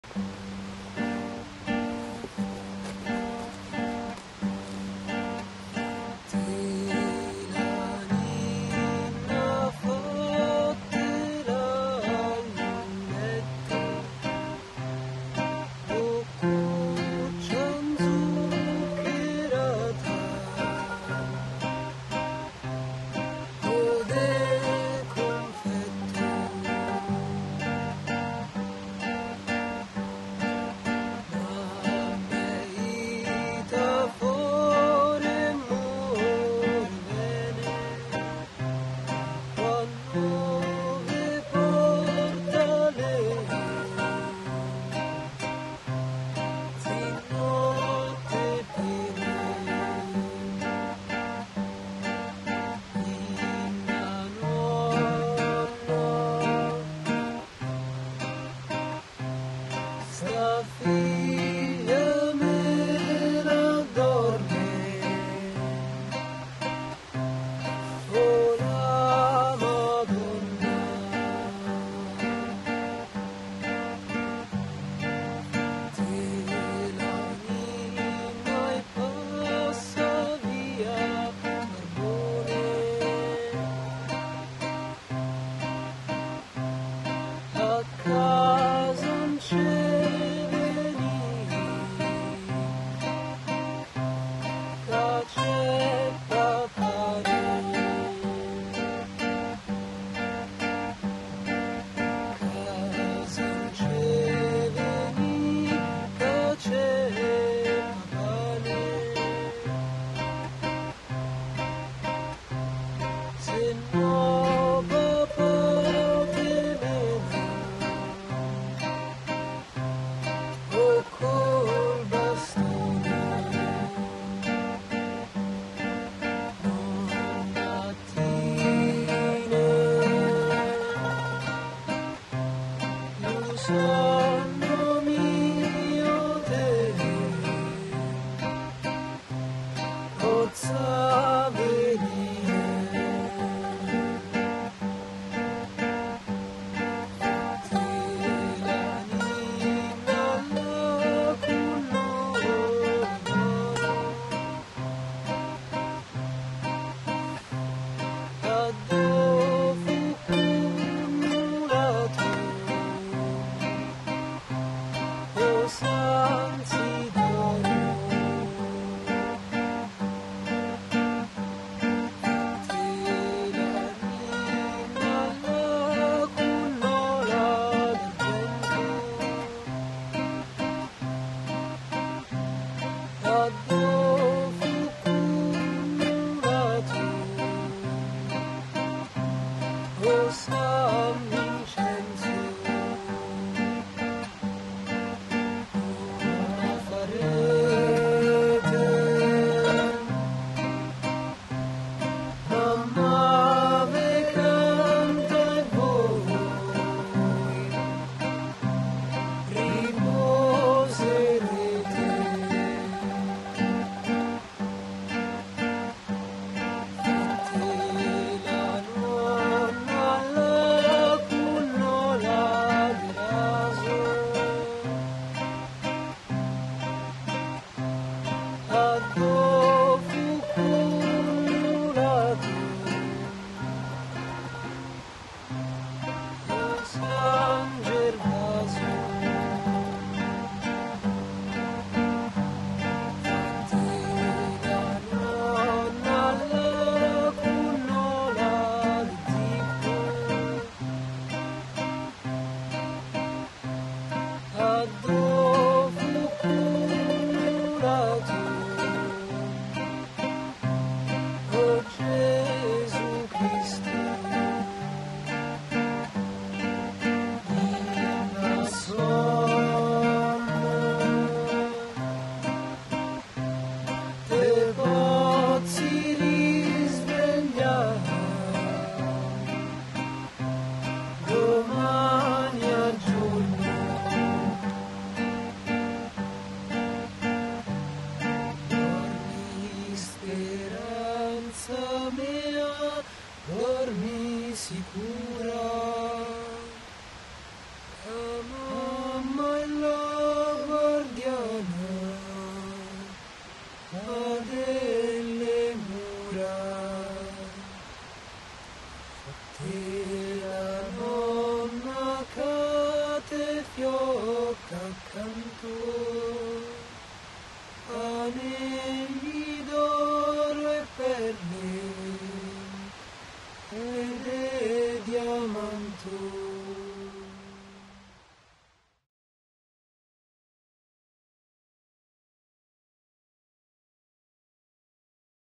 Presepe in arte natura
Allo Schioppo è stato realizzato un presepe arte natura, cioè un presepe che ha utilizzato i materiali presenti il loco, sia naturali che da riciclo. Per chi non è di Frosinone, lo Schioppo è un'area sul fiume Cosa autogestita in regime di Bene Comune da Schioppo Bene Comune, un coordinamento composto da cinque associazioni. Su questa area è stata avviata la procedura per renderla Monumento Naturale, ma è già in atto da tempo una pratica condivisa che agisce come già lo fosse.